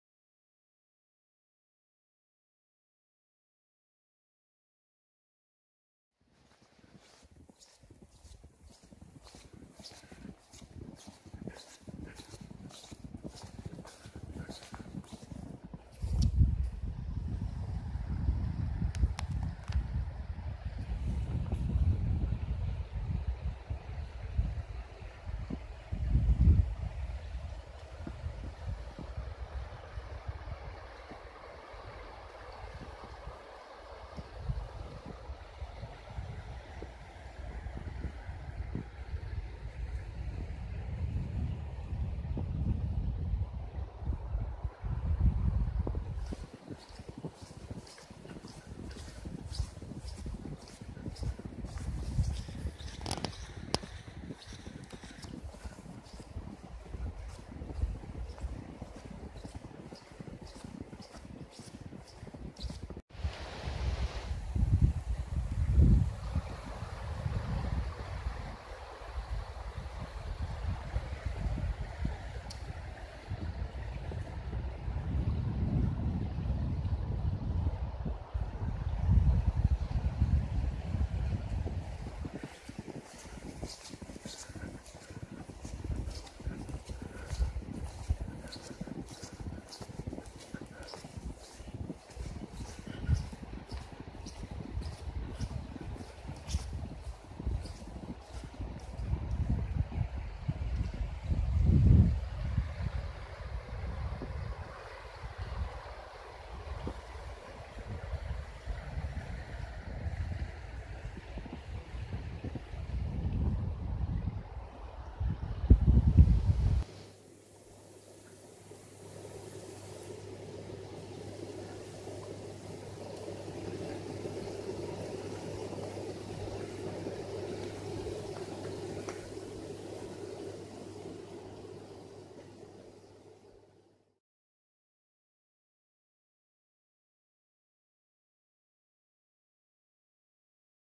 Getting away from it all in Iceland.
Going for a walk in fresh snow by the Eystri-Rangá at Reynifell, nr Keldur and Hella.
Nothing except miles of snow!
I'm actually walking on the F210 road, not that you would know it! I had to turn back after a while, because there were no visual cues as to where the road went (even though I am familiar with it)!! Just white.